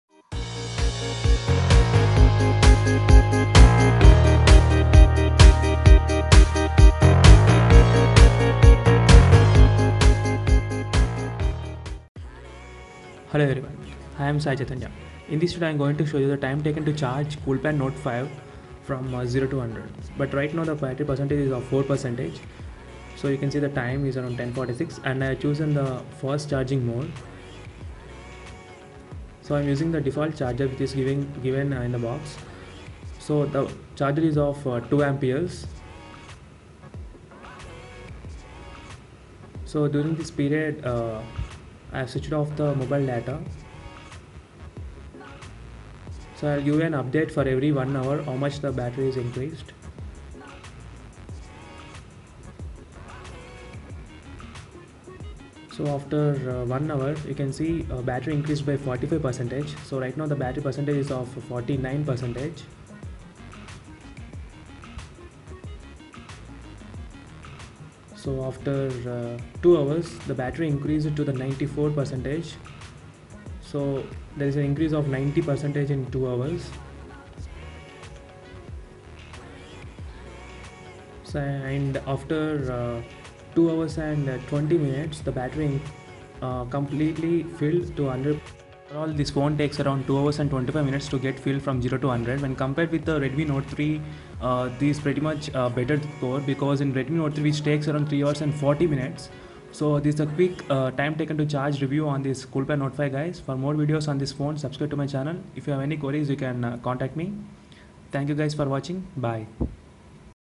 Coolpad note 5 Time Taken to charge 0-100%💥💥💥
Coolpad note 5
13MP primary camera with auto focus and 8MP front facing camera with flash
(5.5-inch) FHD IPS capacitive touchscreen with 1920 x 1080 pixels resolution and 401 ppi pixel density
Android 6.0 + CoolUI operating system
with 1.5 GHz, Qualcomm Snapdragon 617 MSM8952 octa-core, cortex A53, Adreno 405 graphic processor,
4GB RAM, 32GB internal memory expandable up to 64GB and dual SIM (nano+nano (hybrid))
dual-standby (4G+4G) with VoLTE
4010 mAh battery

Best rated cases:-
2.Transparent case
3.Transparent case